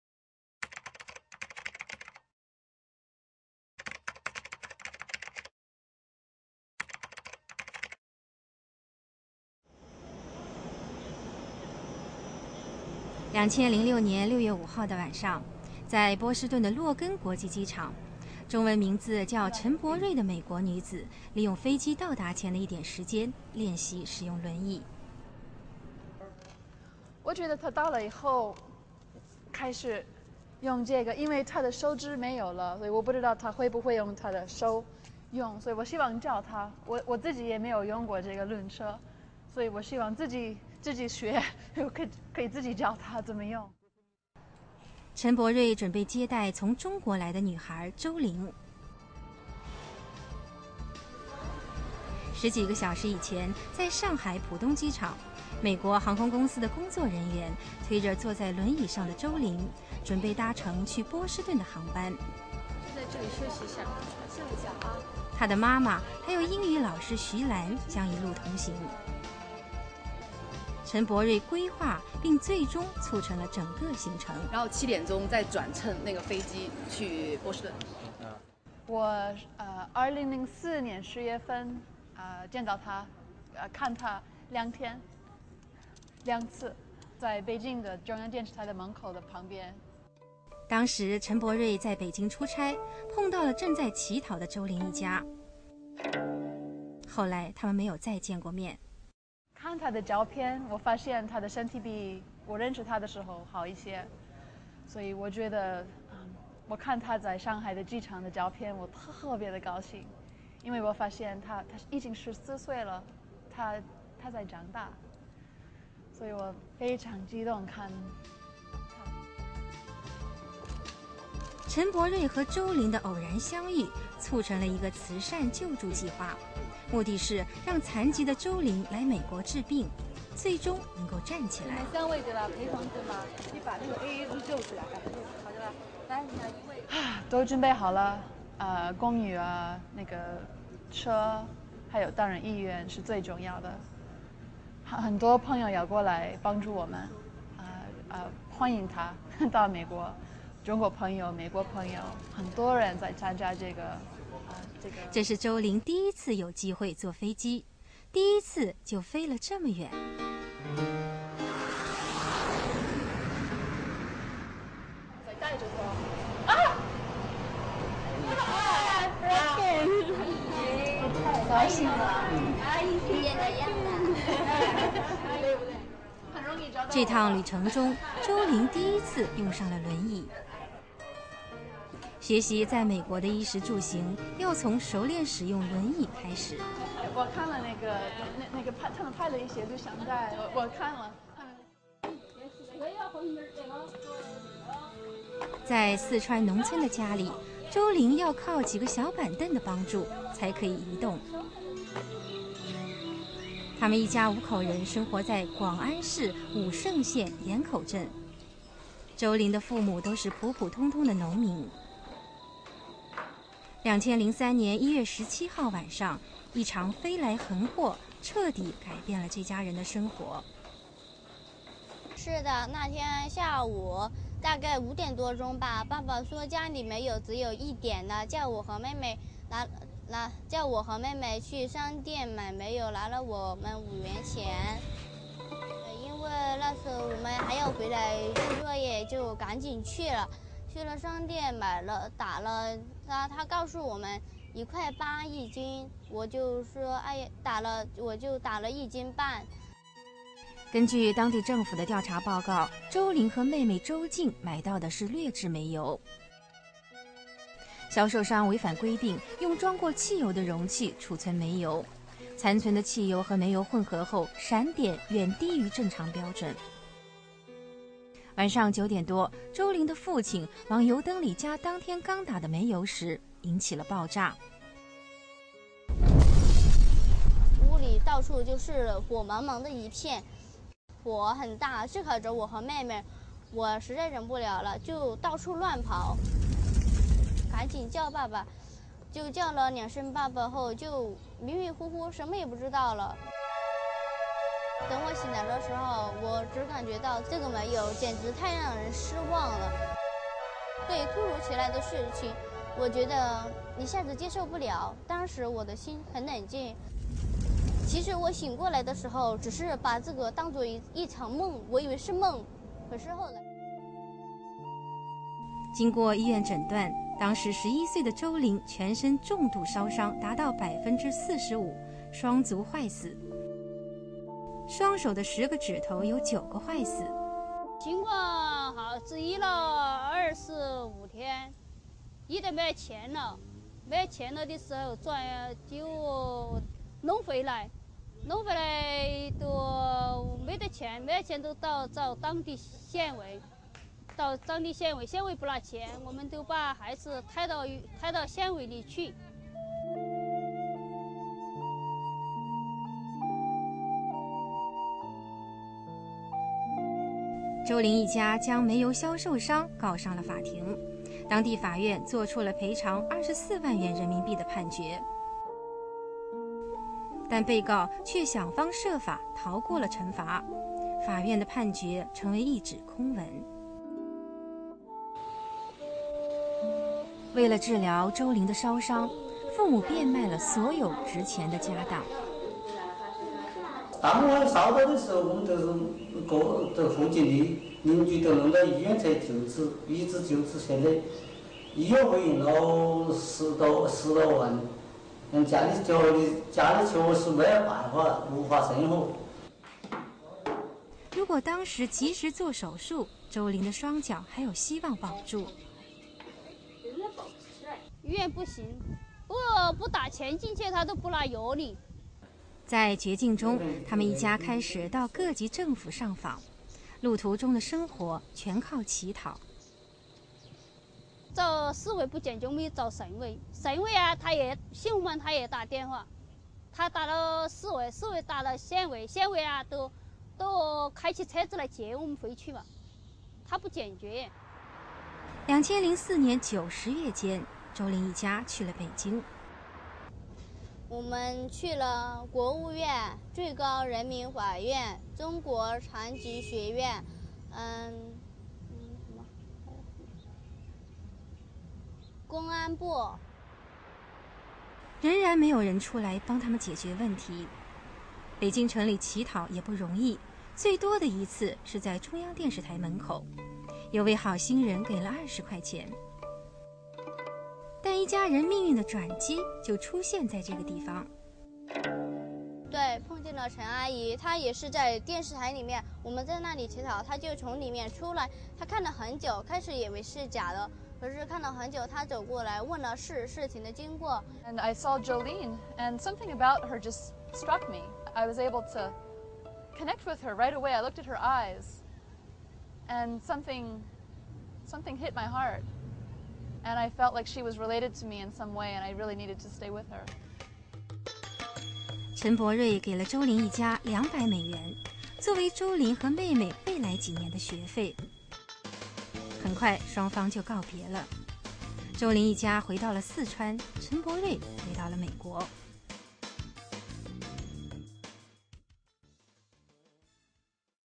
HandReach Story of Zhou Lin Part 01
There are many child trauma patients just like this all over the developing world.  for more information and to donate to the cause.  We welcome any support to help these children receive medical treatment, prosthetic limbs, and rehabilitation so they may live full productive lives.